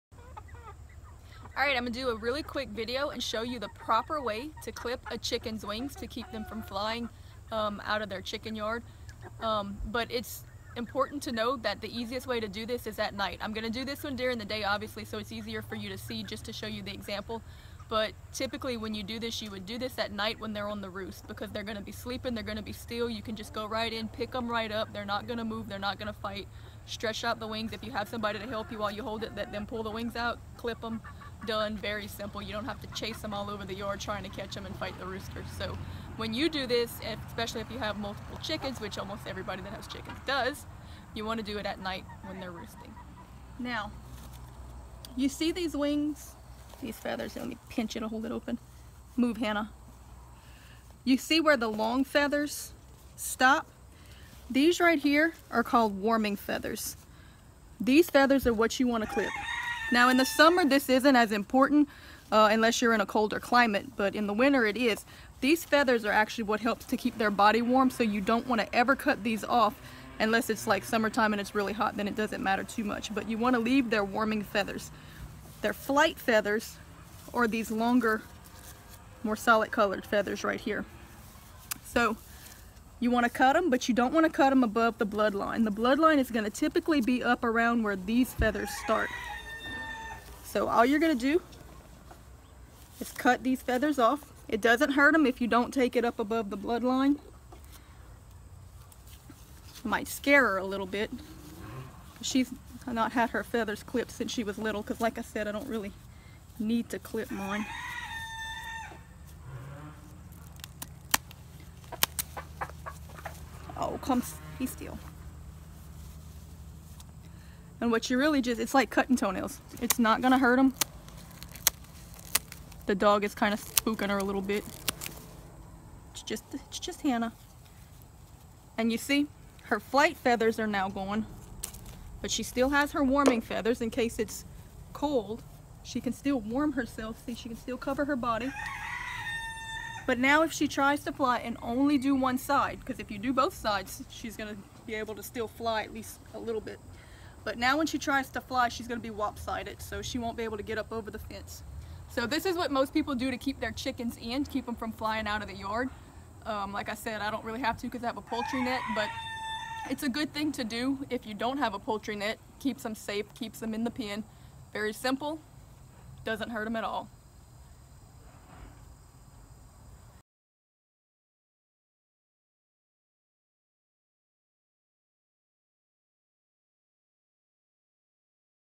The Proper Way to Clip or Trim A Chicken's Wings
Quick and easy, all you need to know guild on clipping your chicken's wings without hurting your chickens so they can't fly out of their enclosures!